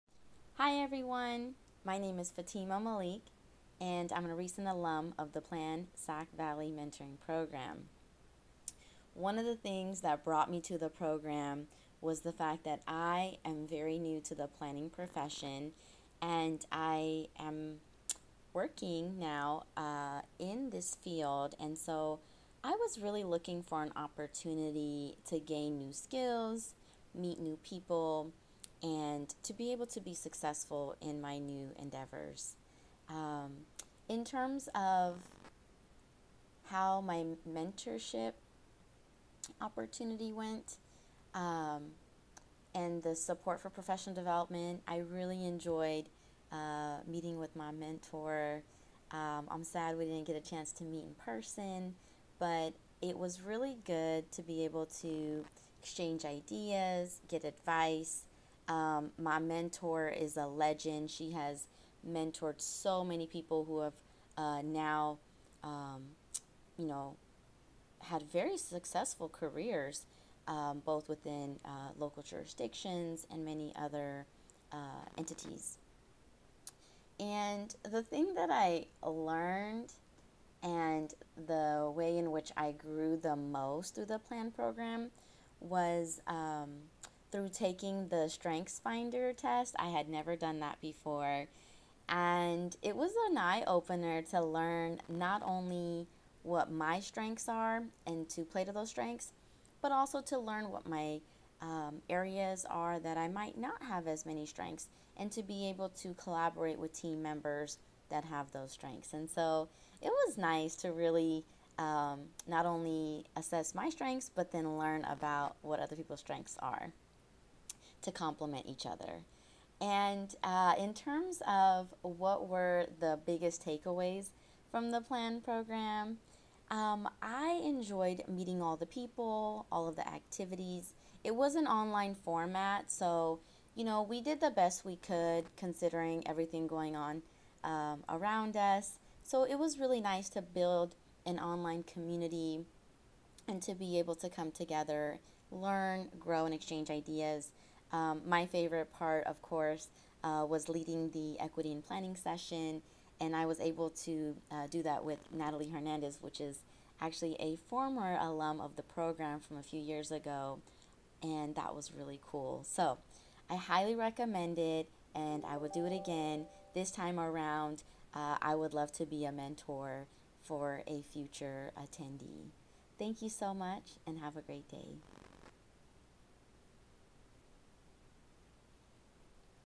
PLAN Interview Fatima Malik
video clip highlights my experience with the 2021 PLAN Sac Valley program